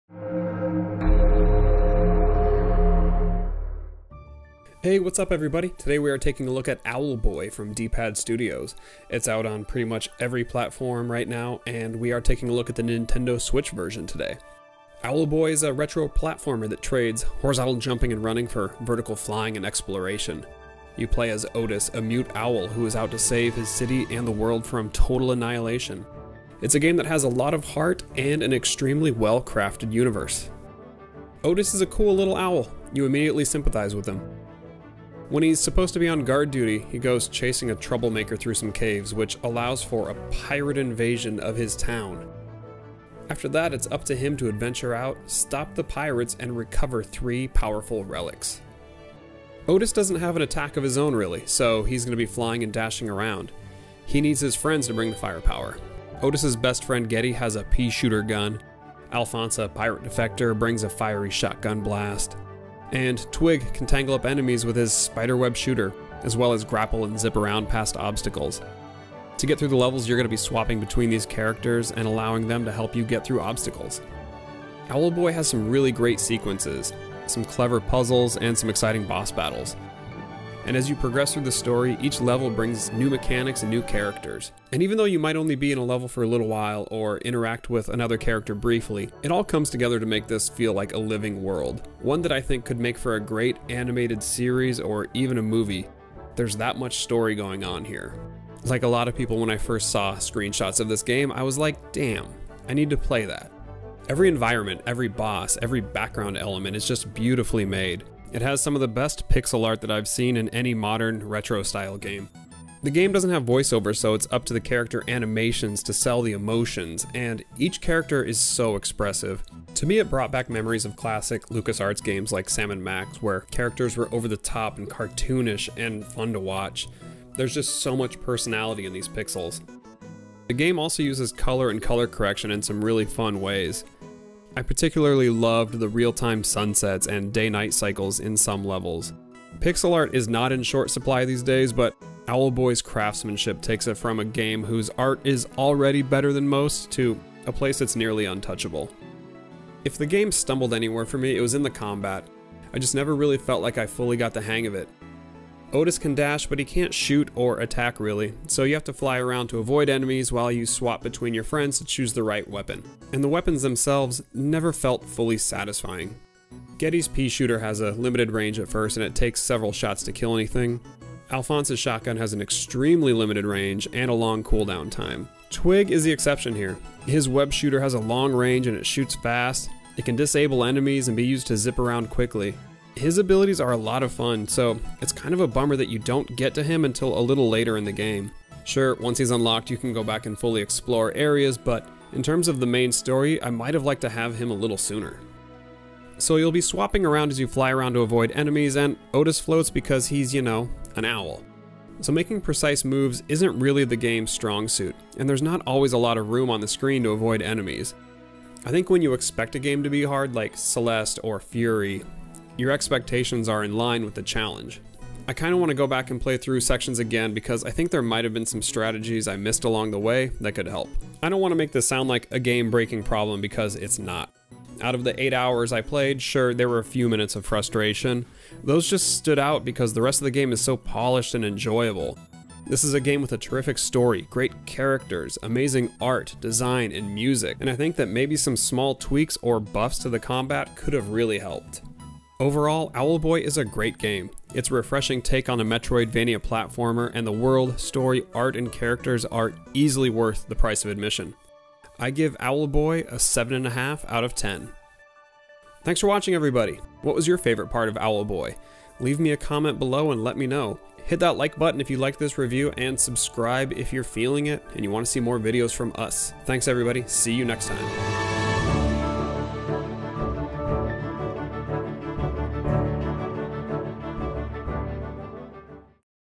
Owlboy - NINTENDO SWITCH - Review
Owlboy is an open-world exploration game with a unique mix of flight and platforming. Instead of running and jumping, you're flying and dashing! Does the game soar in the clouds? Find out! (Developer provided review code)

🕹️ THM GAMING  🎮

Reviews, gameplay, streams, friendships.